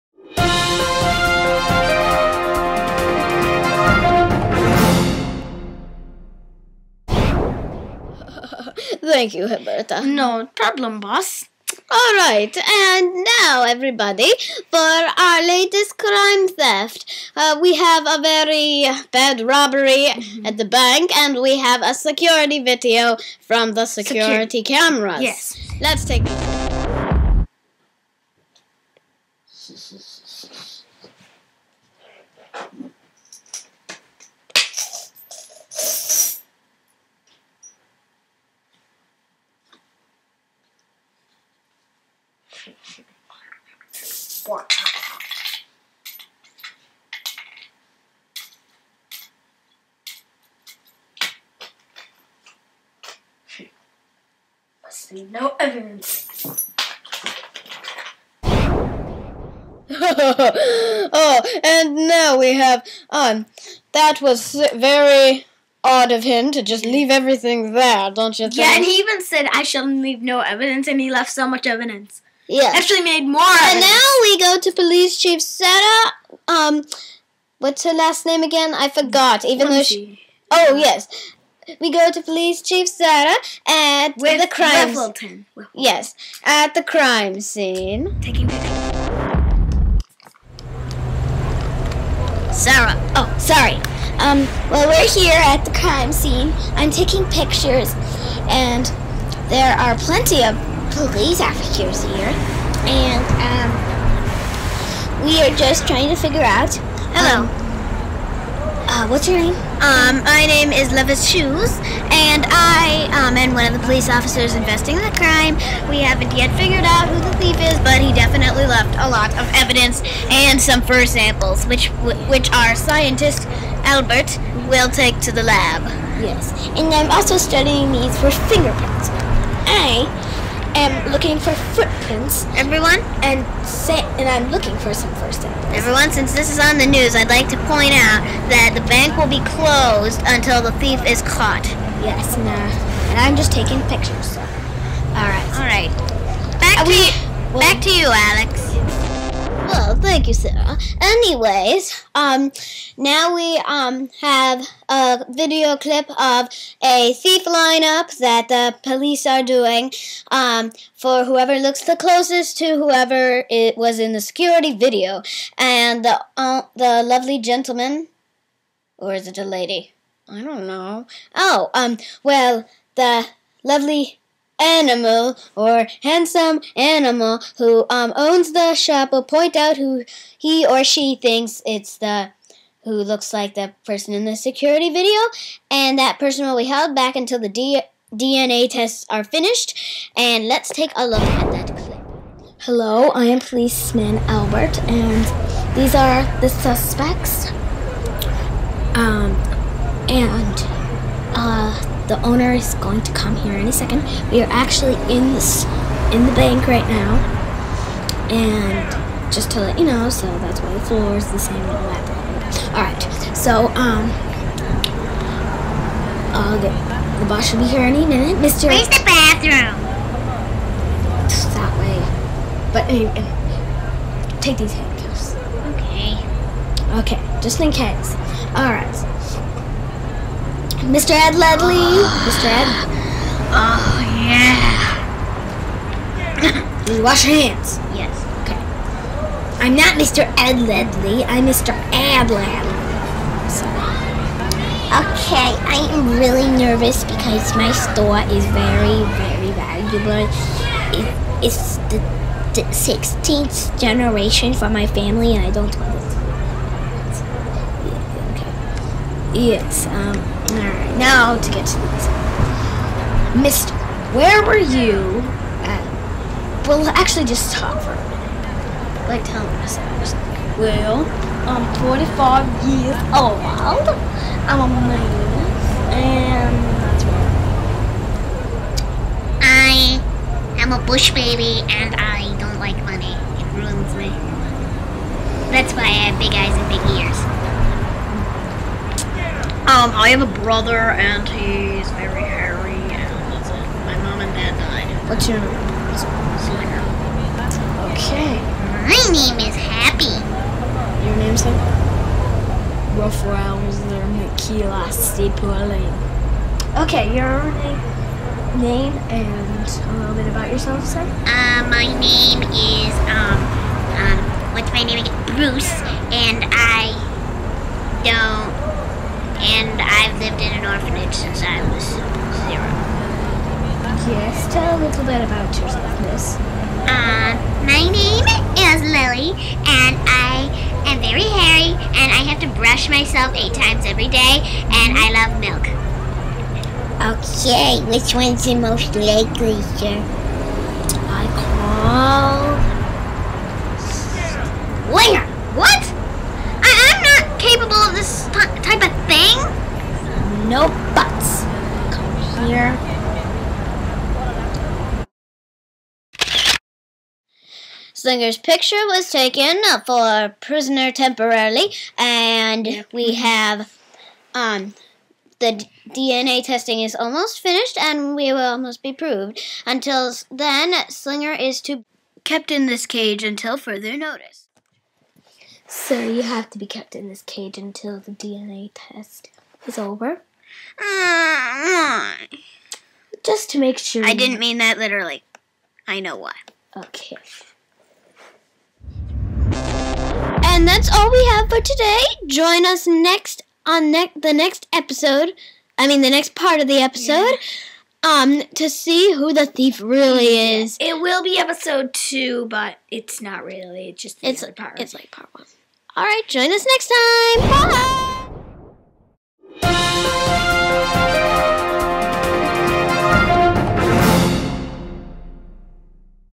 Webkinz News [Episode #1]
Webkinz News with Alex the lion. Enjoy this funny and cool crime bit! ENJOY! :)